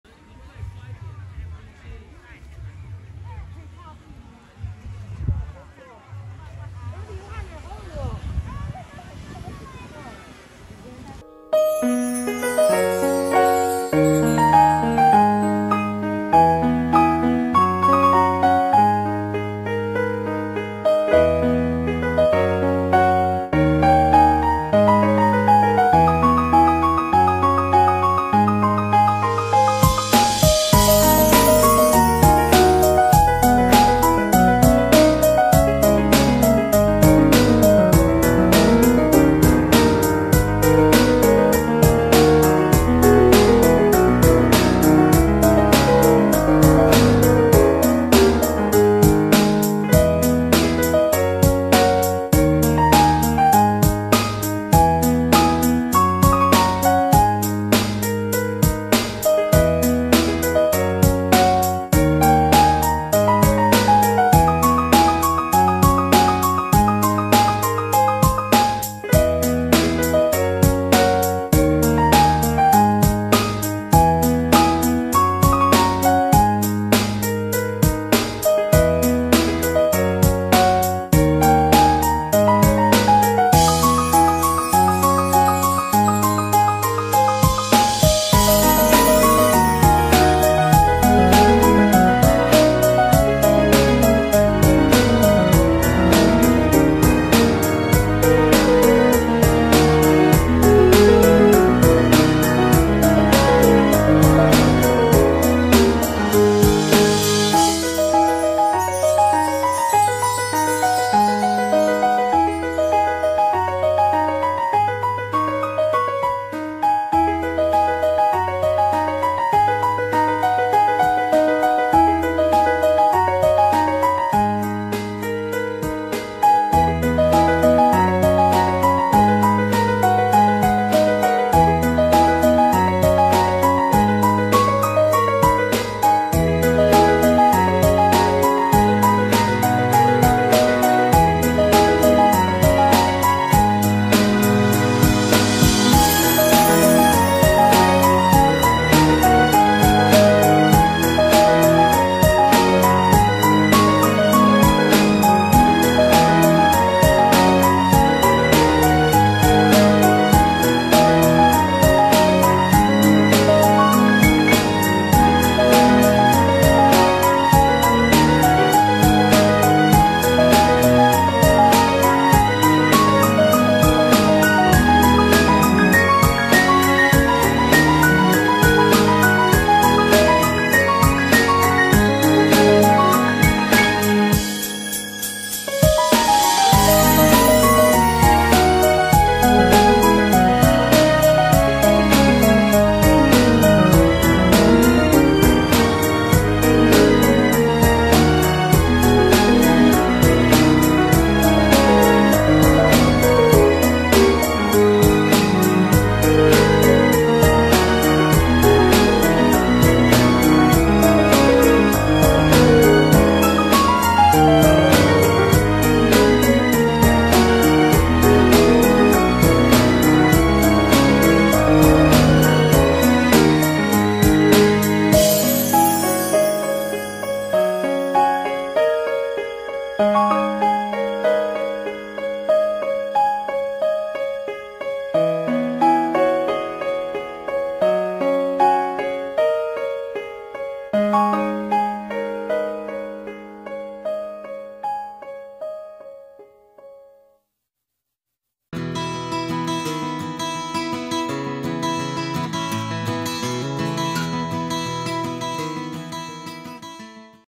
[藍迪愛飛翔(4K)#142]桃園|大溪|月眉|2021花彩節(空拍)
運用農地休耕期間，規劃大面積花海、周邊農事體驗的桃園花彩節，已經成為許多人每年必訪的行程之一！